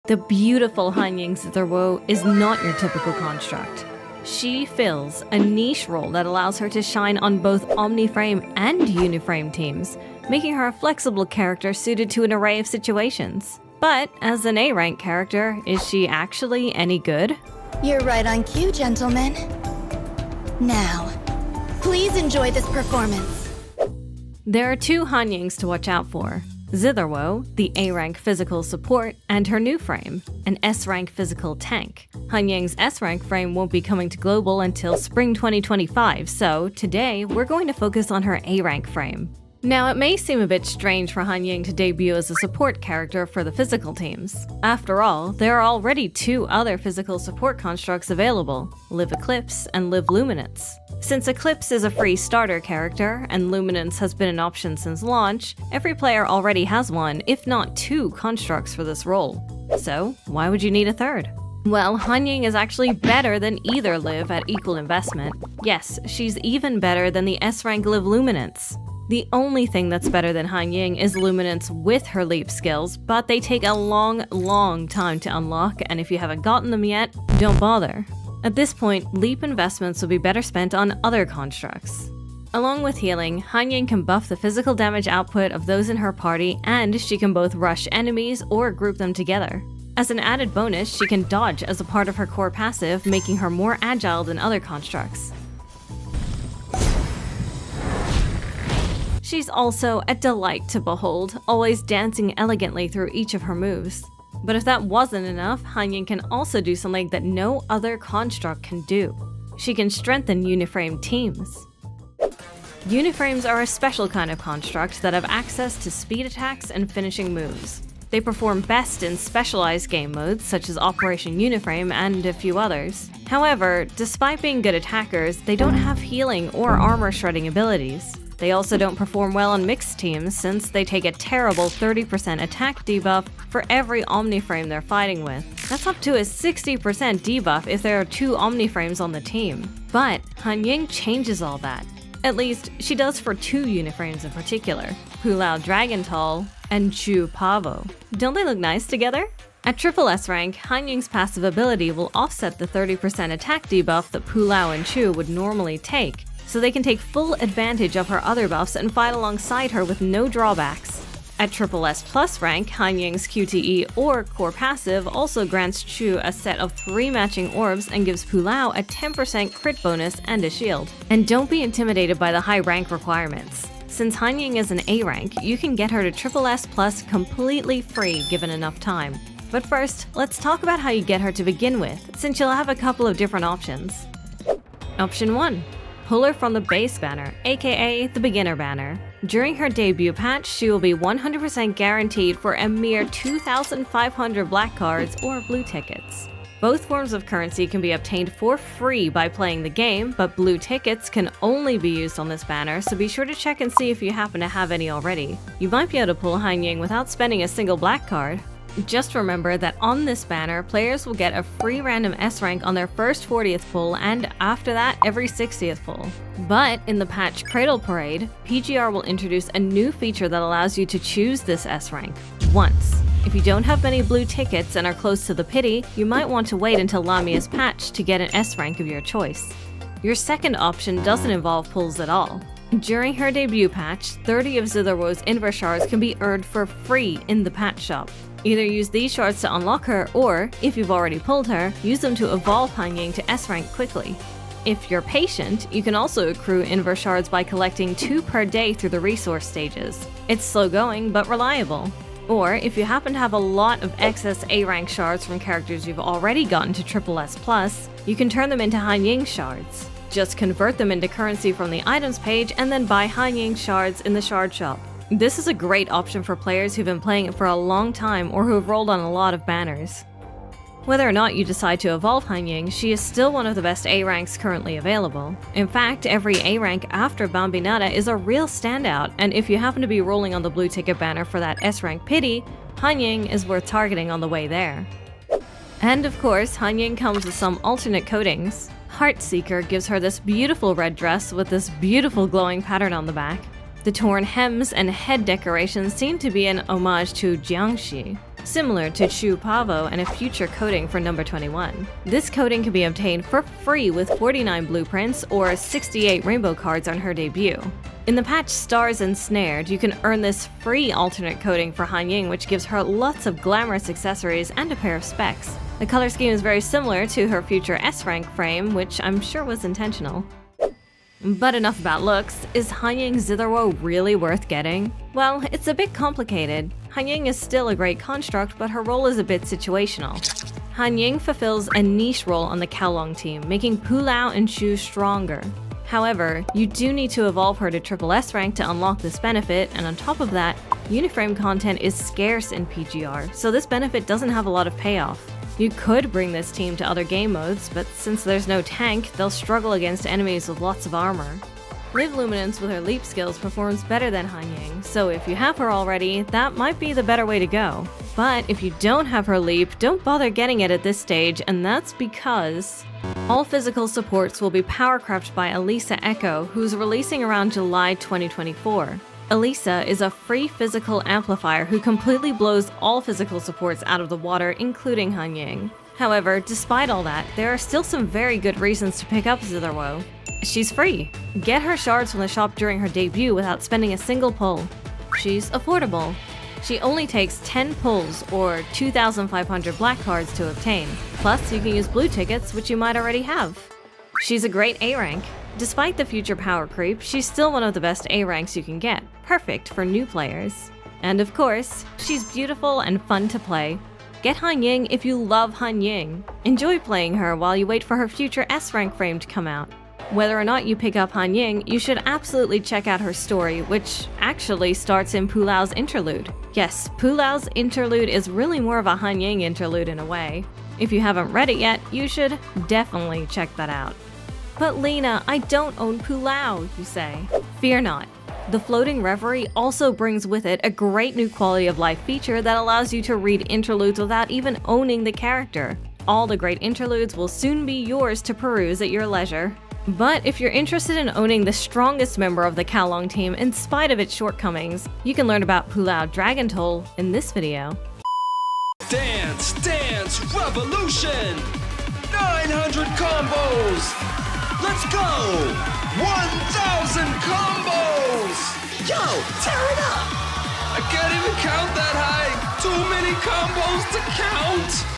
Should you pull Hanying: Zitherwoe?
You can never have too many physical support constructs.

VIDEO EDITORS:
♦ Ivy & Lina

SCRIPT:
♦ Ivy & Lina

VOICE OVER:
♦ Lina

▶ CHAPTERS
0:00 The META
1:43 Her Niche
2:58 How to get and evolve her (for free!)
4:45 Alternate outfits
5:20 Pros and Cons
6:50 Semi-hidden lore
7:28 (See you in the arcade!)